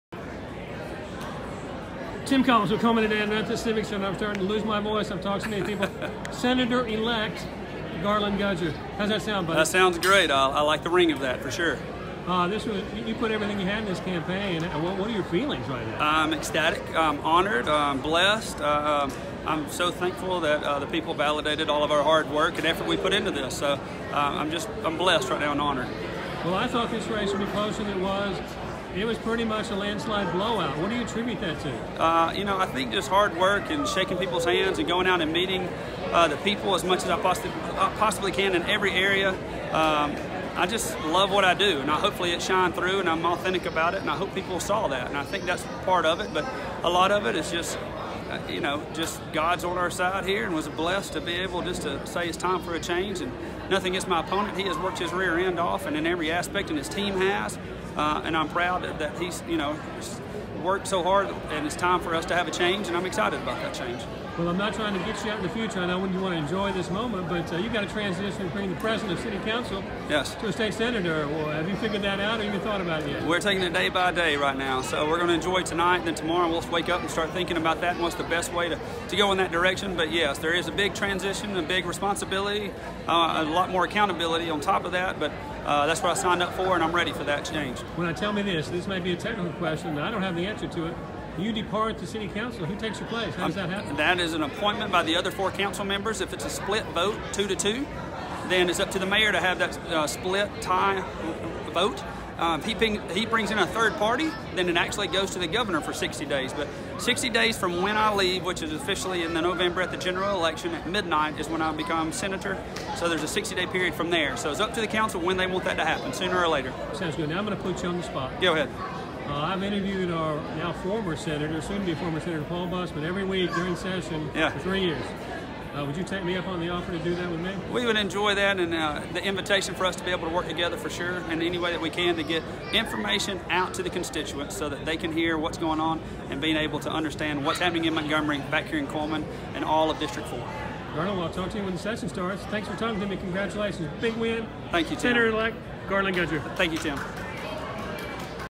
NEW District 4 Senator-Elect: Garlan Gudger
★ GARLAN GUDGER ★

Final Unofficial Election Results (59-41)

Garlan Gudger Jr. defeats 2-term incumbent State Senator Paul Bussman by a near landslide margin.

in ALL Counties including Lawrence, Marion, & Winston:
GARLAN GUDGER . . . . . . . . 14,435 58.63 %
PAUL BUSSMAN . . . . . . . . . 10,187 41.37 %

in Cullman County:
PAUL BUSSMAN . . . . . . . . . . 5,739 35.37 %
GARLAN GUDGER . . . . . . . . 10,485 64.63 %

in Lawrence County:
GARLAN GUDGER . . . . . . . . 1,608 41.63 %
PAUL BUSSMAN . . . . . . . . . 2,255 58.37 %

in Marion County:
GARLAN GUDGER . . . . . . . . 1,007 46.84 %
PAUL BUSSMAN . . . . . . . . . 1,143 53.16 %

in Winston County:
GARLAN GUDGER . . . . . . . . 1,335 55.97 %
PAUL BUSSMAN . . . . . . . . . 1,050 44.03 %